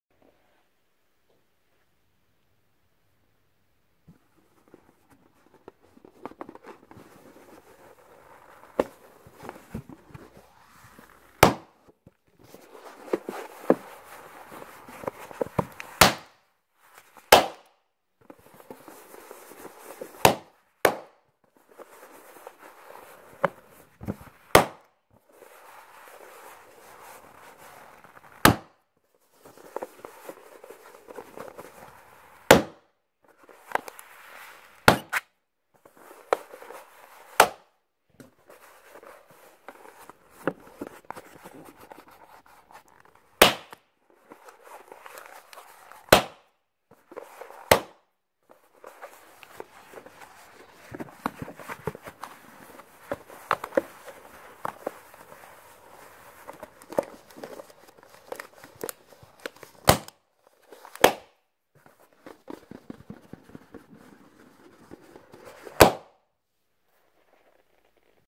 Happy New Year! | Balloon popping (small) with hands | ASMR
G’day and welcome! In this video we’ll explore the sounds made by popping small water balloons with hands. This video contains some loud sounds. Relax, enjoy and tingle away!
Wishing you all a wonderful 2024!